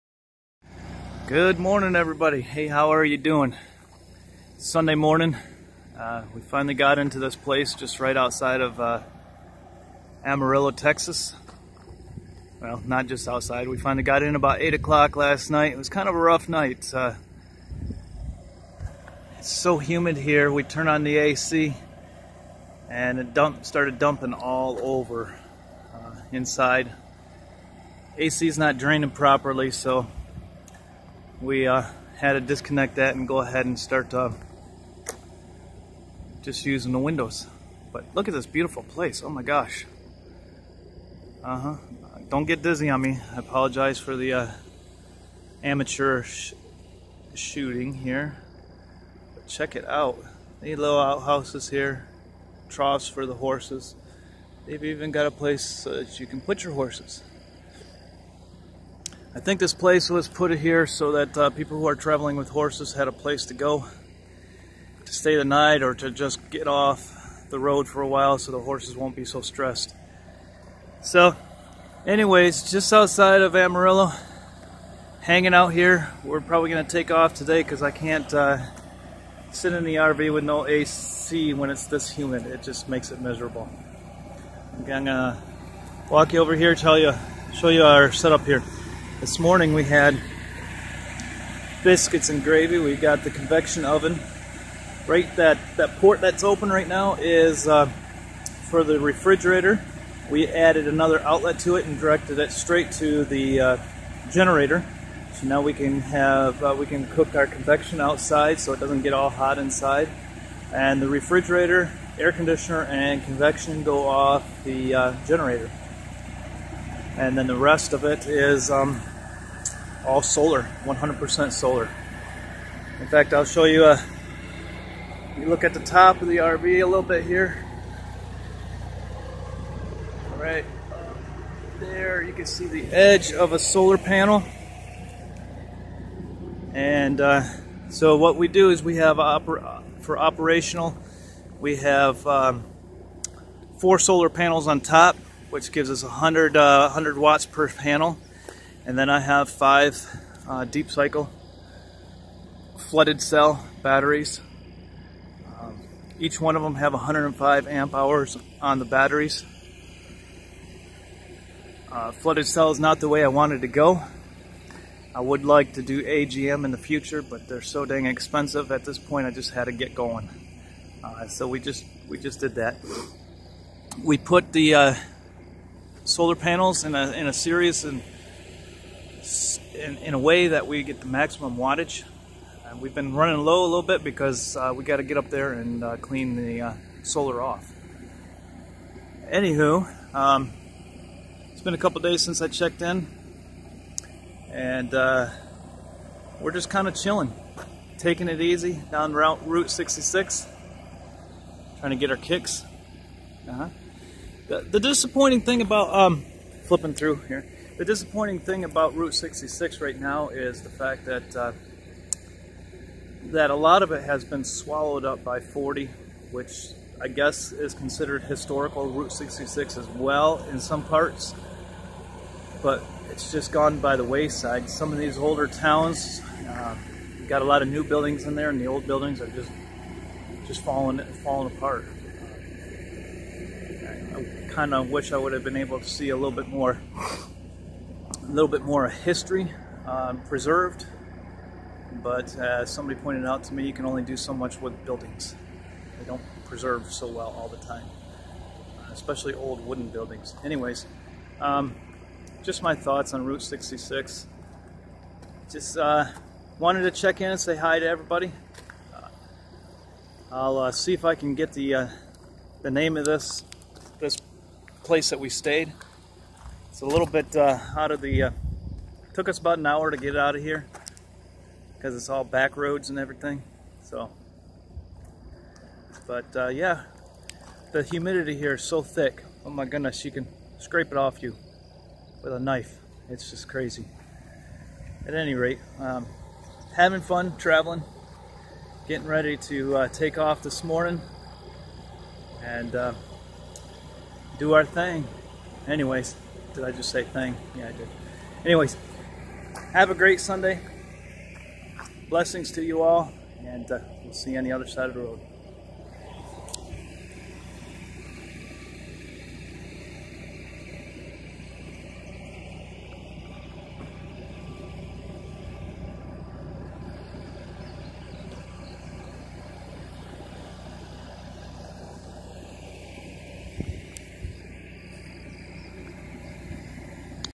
RV living, Texas boondocking.
We are currently in Texas having AC problems. But still having fun.